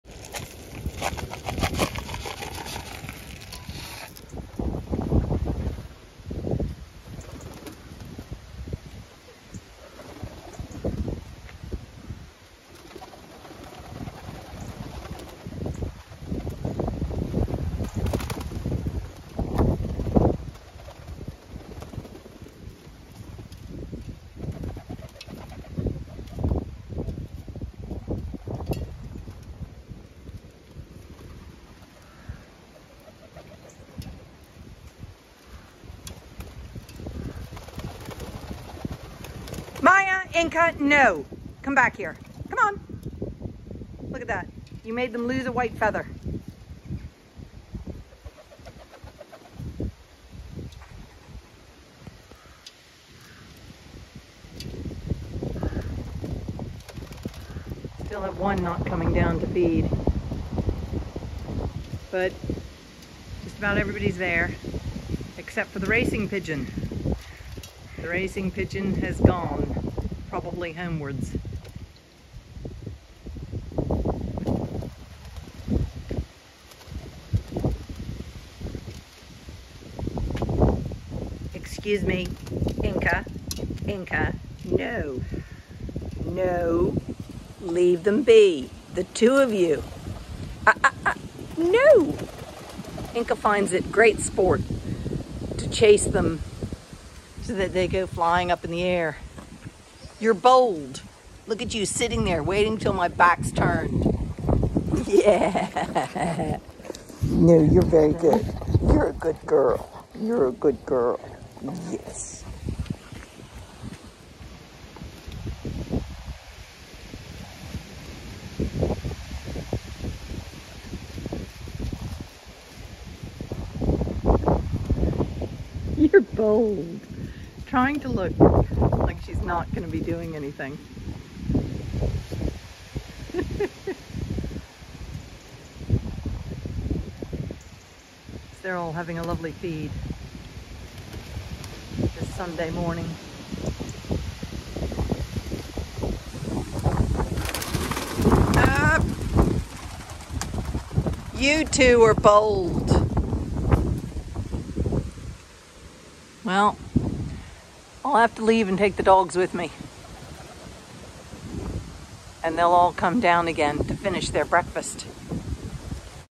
Doves breakfast interrupted by bold dogs, Maya & Inca
I love the doves cooing in the yard. They’ve their own roosts to sit on inside the stables out of the weather and away from birds of prey. The bold dogs get great pleaser in making them fly up in a flurry of feathers.